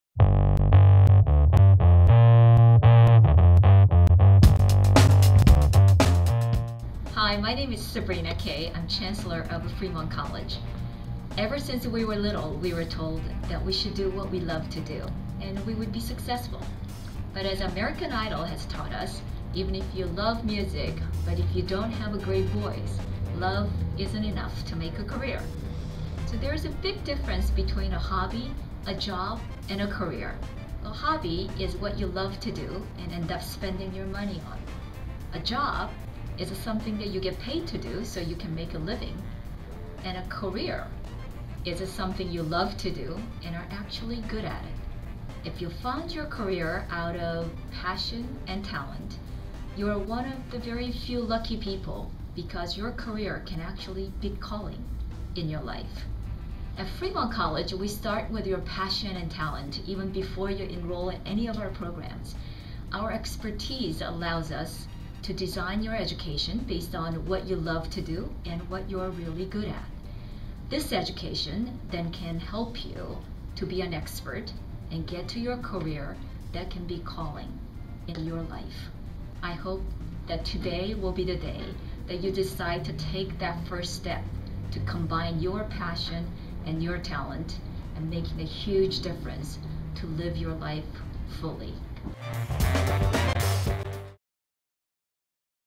Fremont College - The Difference Between a Hobby, Job, and Career - Vlog by Dr. Sabrina Kay
Fremont College Chancellor Dr. Sabrina Kay talks about the difference between having a hobby, job, and career. Fremont College prepares students by focusing on their passions and talents to help them achieve the career of their dreams.